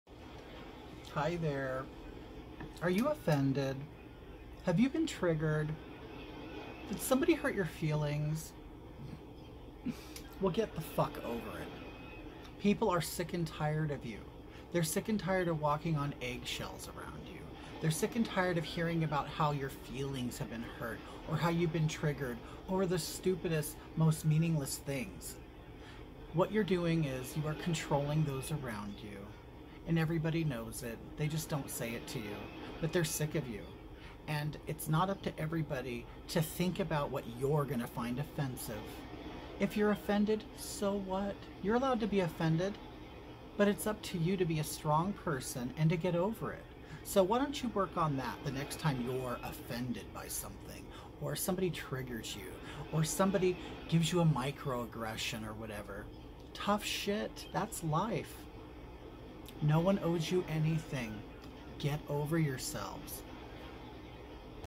Trigger warnings and microaggressions
A discussion on how to handle trigger warnings and being offended